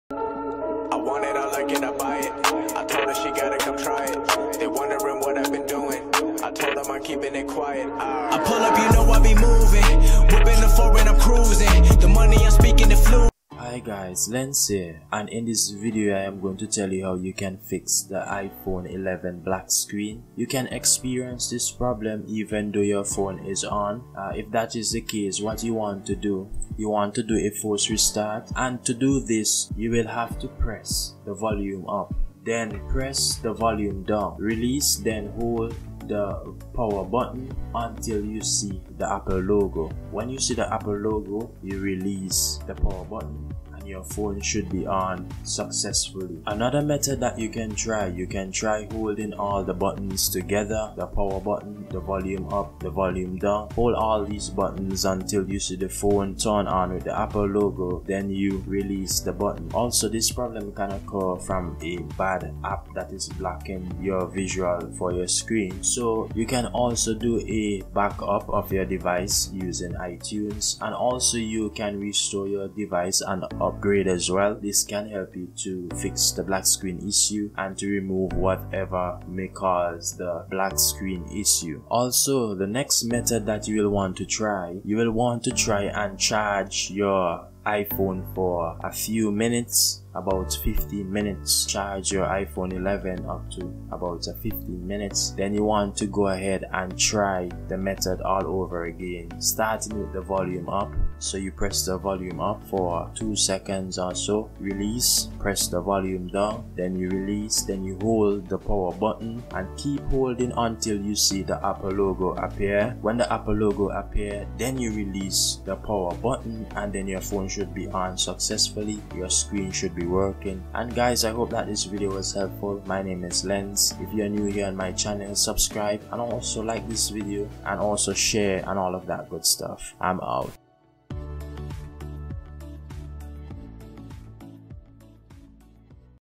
How to fix iphone 11/11 pro max Xr black screen-iphone 11 black screen
Press volume up release
press volume down release
press and hold power button until you see logo.
Release